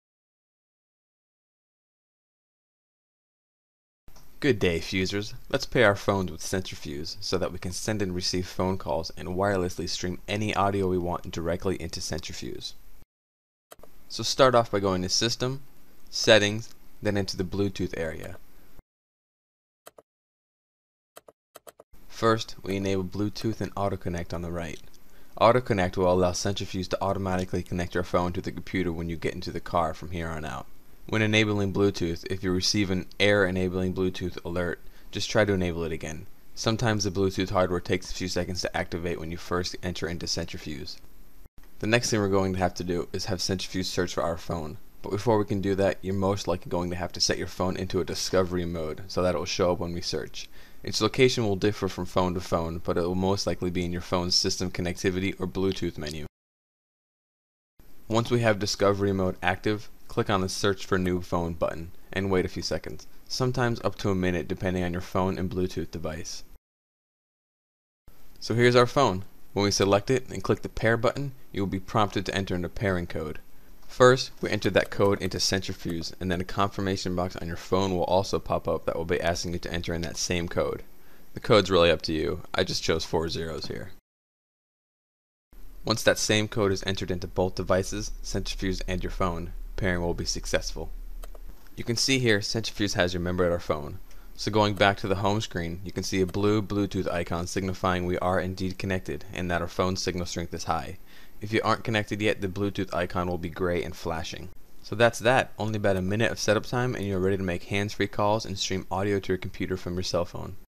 Centrafuse - Pairing your Bluetooth Device or Cell Phone to Centrafuse
By pairing your Bluetooth Device or Cell Phone within Centrafuse you can easily stream music from your device to your computer, or make hands-free phone calls.